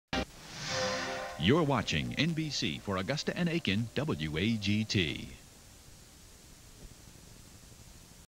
WAGT Station ID 1993-94
This is WAGT's station ID from NBC's 1993-94 season "The Stars Are Back on NBC"
WAGT was located in Augusta, GA